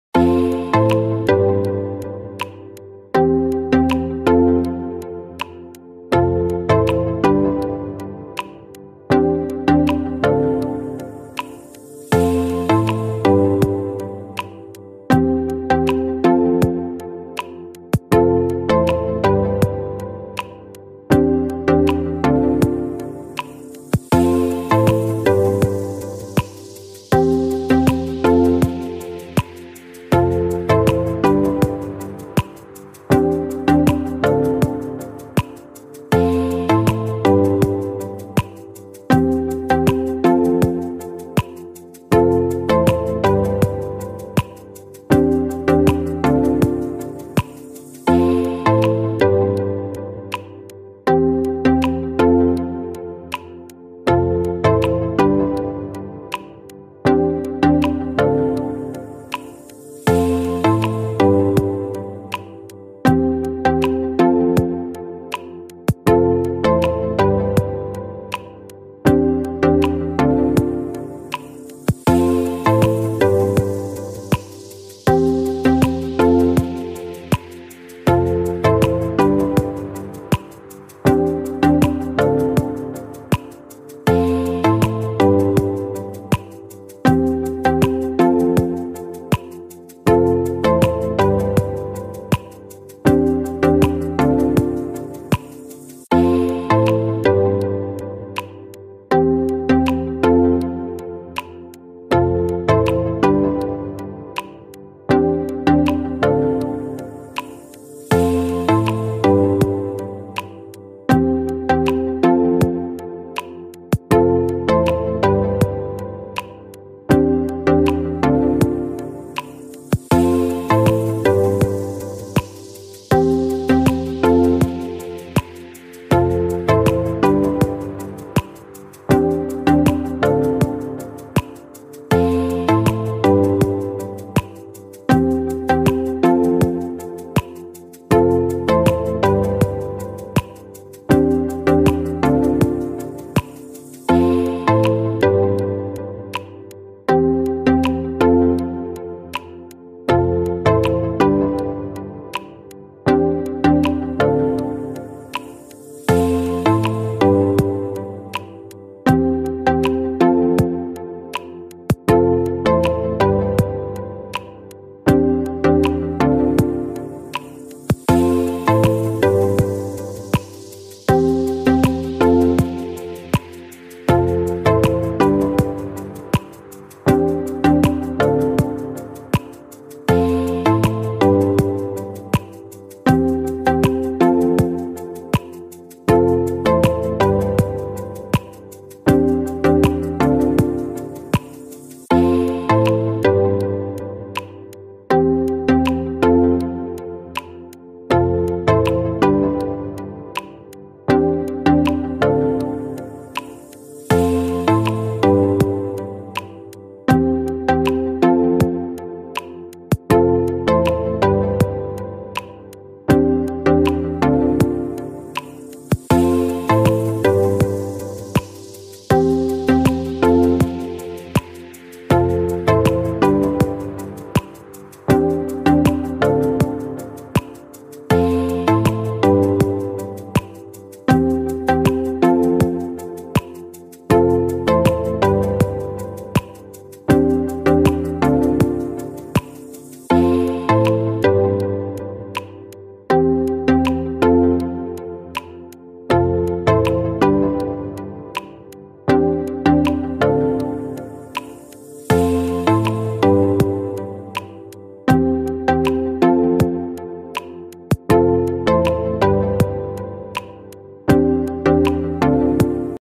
Decoupage wooden spoons/ Decoupage for beginners
We are decoupaging wooden spoons for decorative purposes. A great beginner project.
Any questions please ask below😁